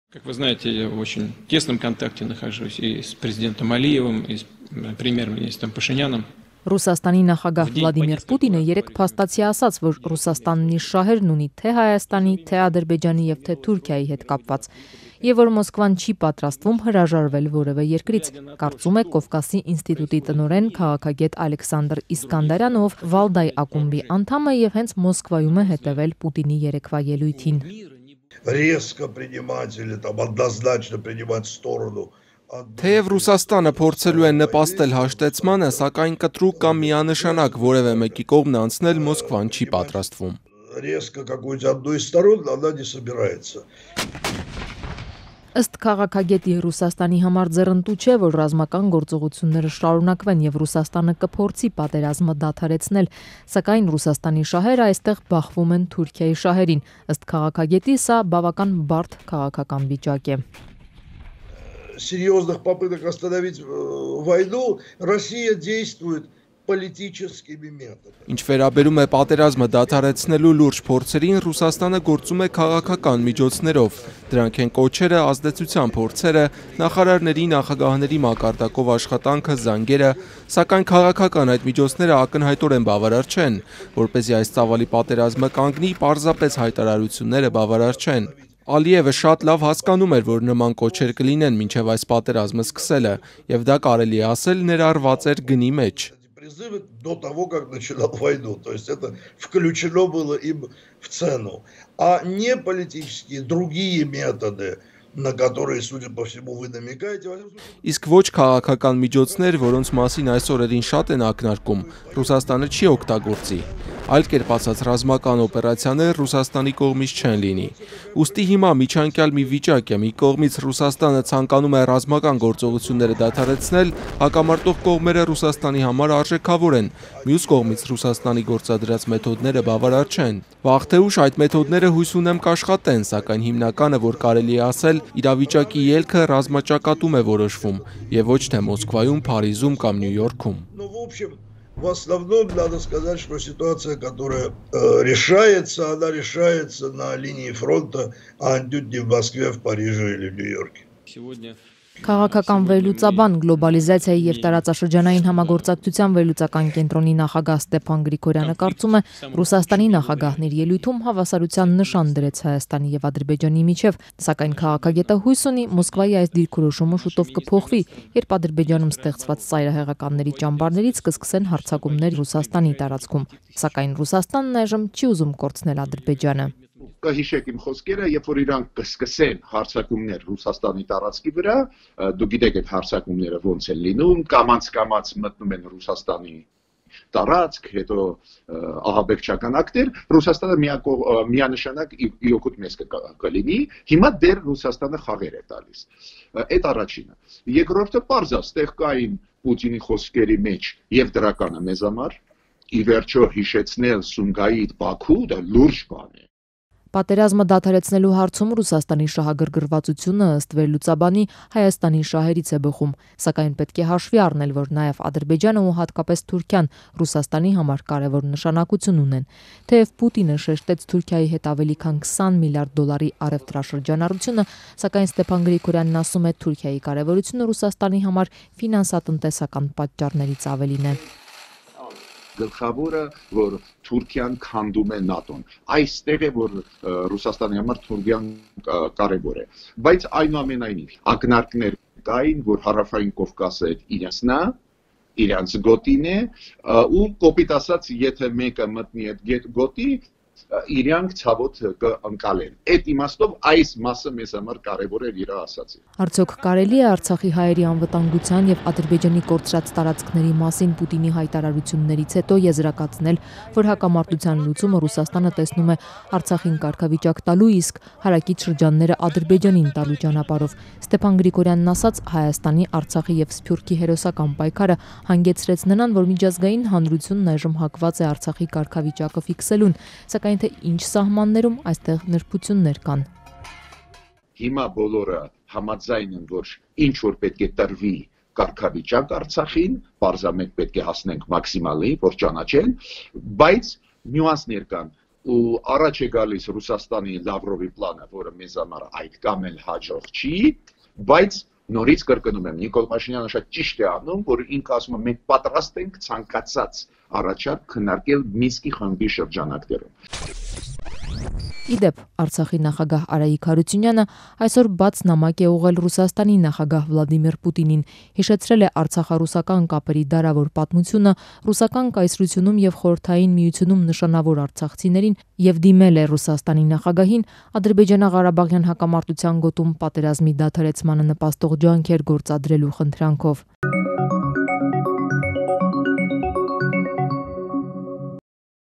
ՌԴ-ն, քաղաքագետի կարծիքով, փորձում է պատերազմը դադարեցնել քաղաքական մեթոդներով, որոնք բավարար չեն
Կովկասի ինստիտուտի տնօրեն, քաղաքագետ Ալեքսանդի Իսկանդարյանի կարծիքով, Ռուսաստանի նախագահ Վլադիմիր Պուտինը երեկ փաստացի ասաց, որ Ռուսաստանն իր շահերն ունի թե՛ Հայաստանի, թե՛ Ադրբեջանի և թե՛ Թուրքիայի հետ կապված, և որ Մոսկվան չի պատրաստվում հրաժարվել որեւէ երկրից։